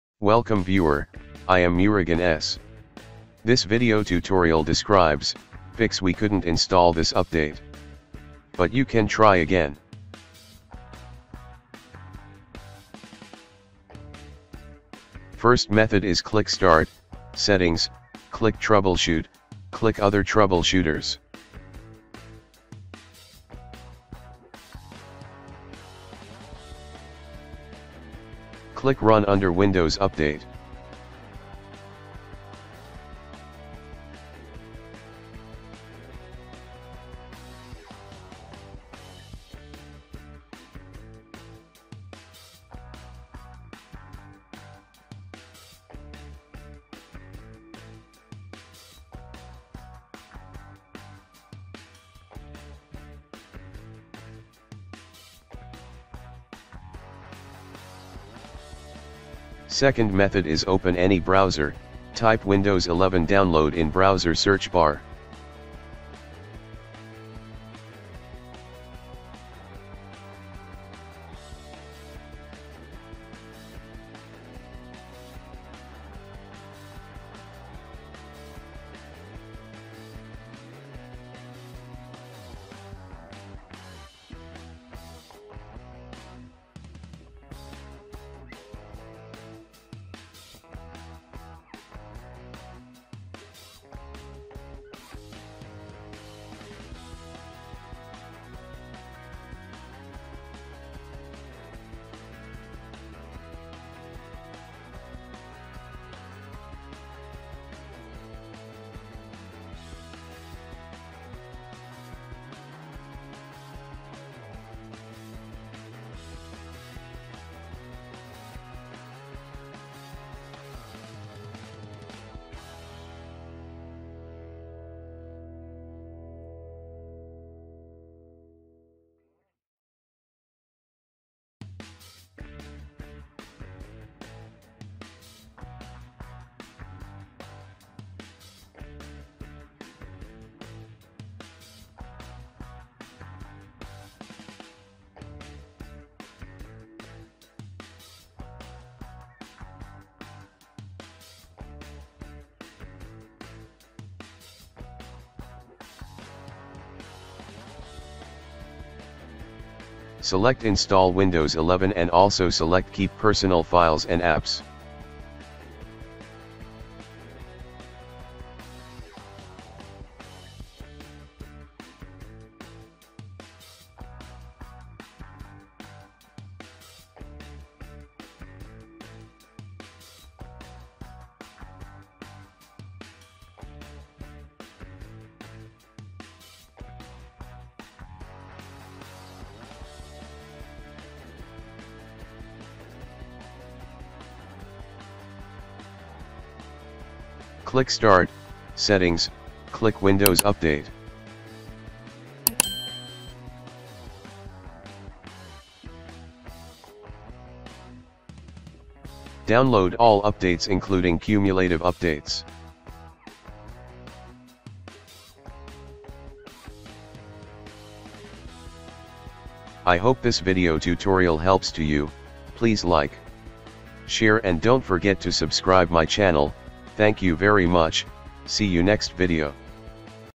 Fix We Couldn't Install This Update. But You Can Try Again (0x8024a22d) In Windows 11
This Tutorial Helps to Fix We Couldn't Install This Update. But You Can Try Again (0x8024a22d) In Windows 11

00:00 Intro
00:18 Method 1 - Update Troubleshooting
00:59 Method 2 - Create Windows 11 Installation Media
03:39 Closing